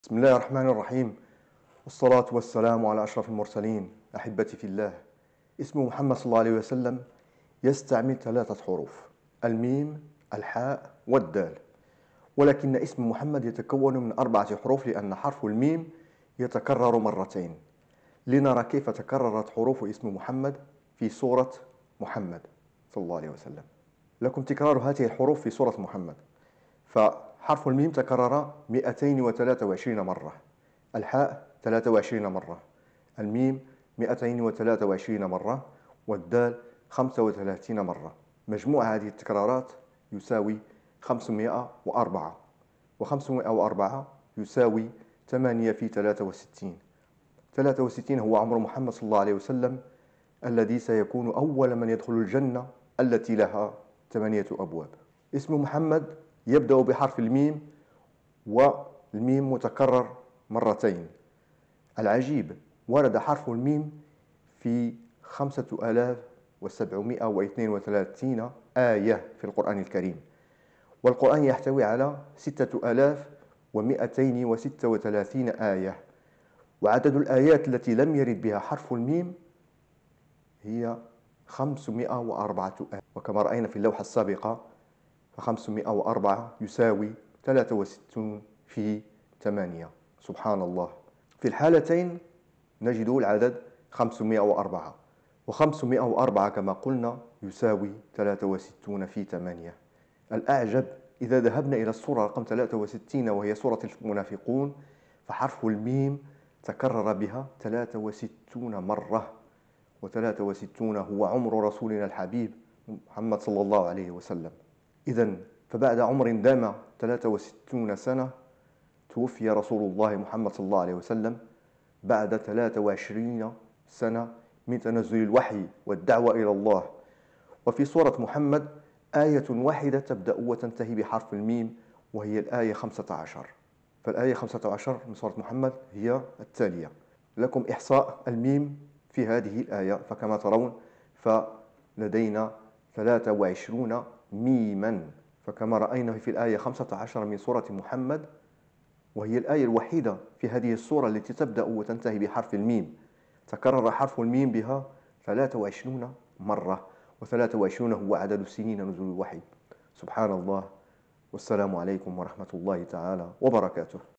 سبحان الله : روابط رقمية عجيبة بين أحرف اسم محمد وعمره صلى الله عليه وسلم
القرآن المعجزة الخالدة : روابط رقمية لا تصدق في القرآن بين حروف اسم  "محمد"، "الميم"، "الحاء" و"الدال"  وعمر رسولنا الكريم.